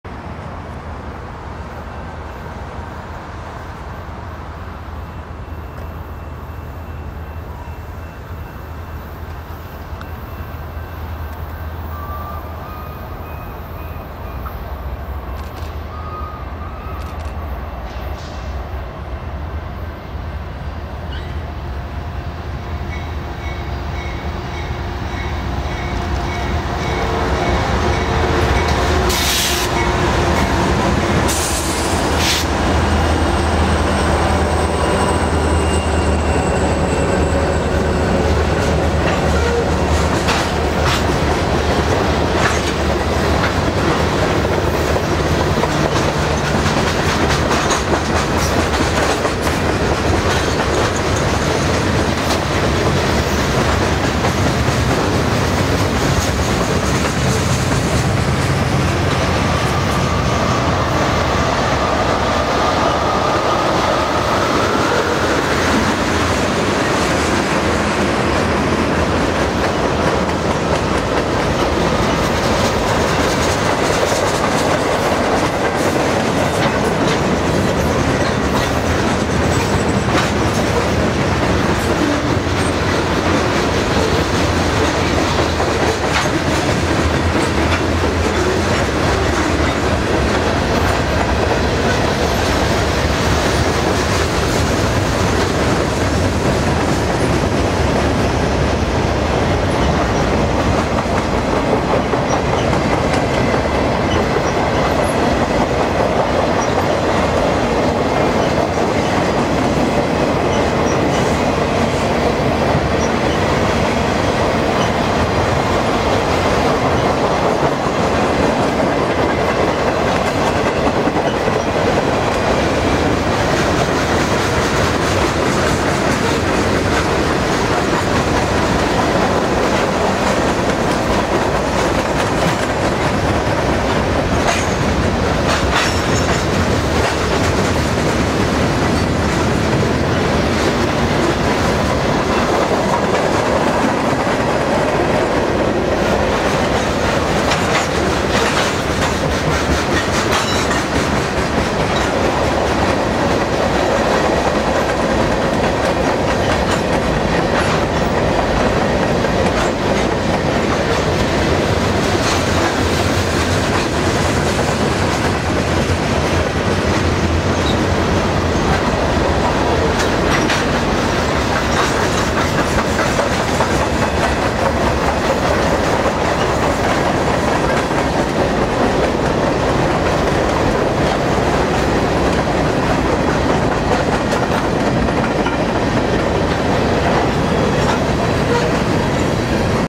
BNSF 5301 @ Old Town Tacoma, WA w Canon HF11
BNSF 5301, 4380, 665, 1831, and 1828 lead a northbound mixed freight through Old Town Tacoma, WA on BNSF's Seattle Sub. A 28 April 2012 video.